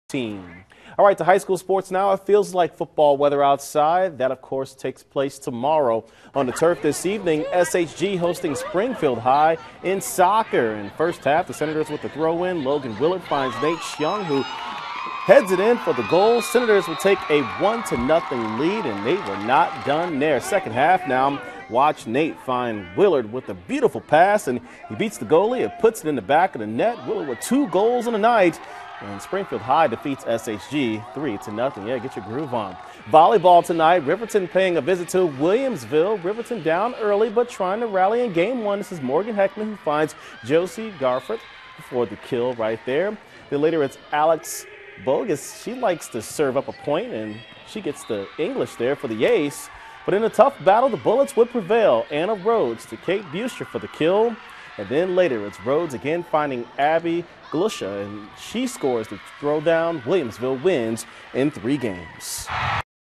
Prep Scoreboard 9-11-14
Highlights of Williamsville over Riverton in volleyball and Springfield High's 3-0 shutout of Sacred Heart-Griffin on the soccer field.